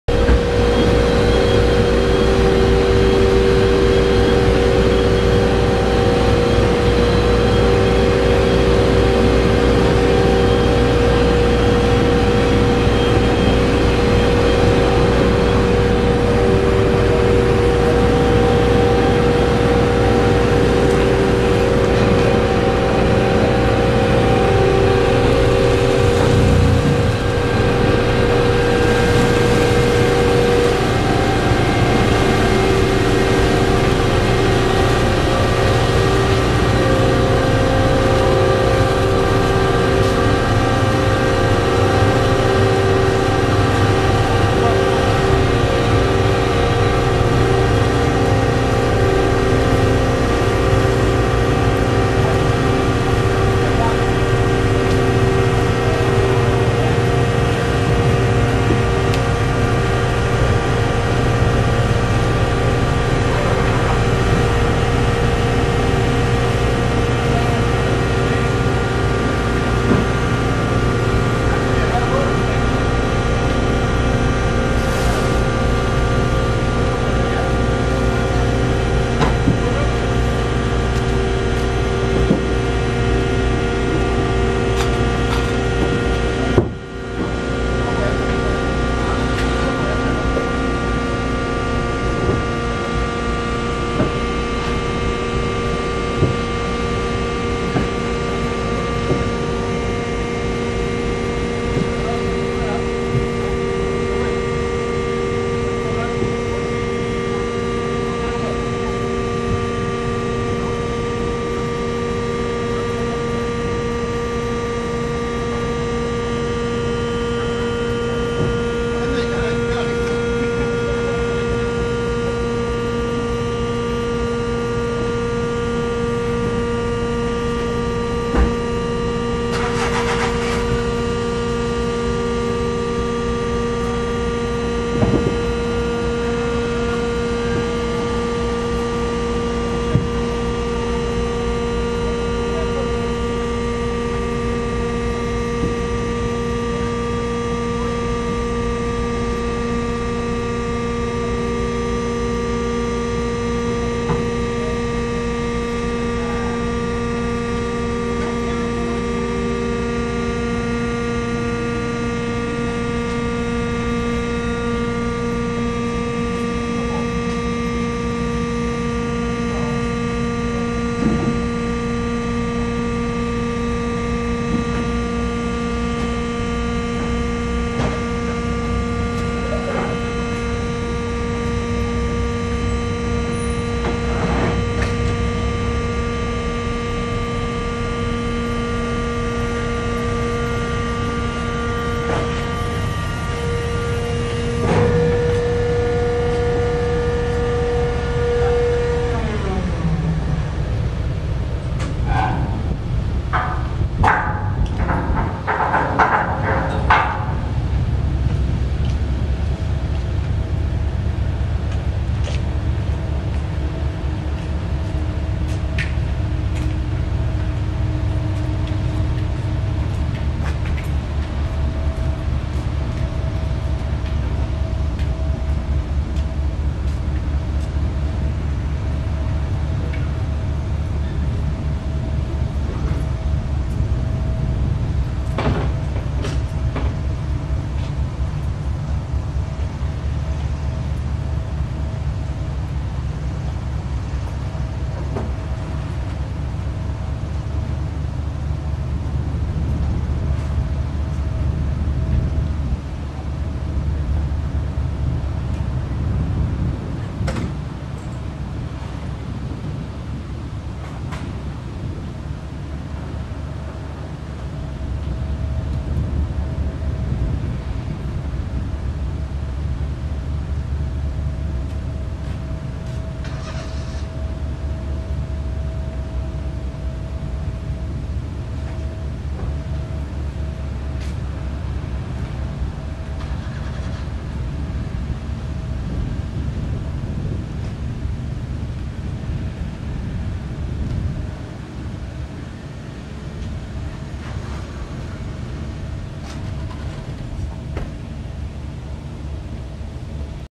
里港の岸壁を離れるフェリー
鹿児島県下甑島の長浜港に向けて、上甑島の里港を出航する甑島商船のフェリー「ニューこしき」です。
２０１４年７月２８日　１８：０５頃

この動画が使われているサイトは「鹿児島県の風景」の「フェリー『ニューこしき』」です。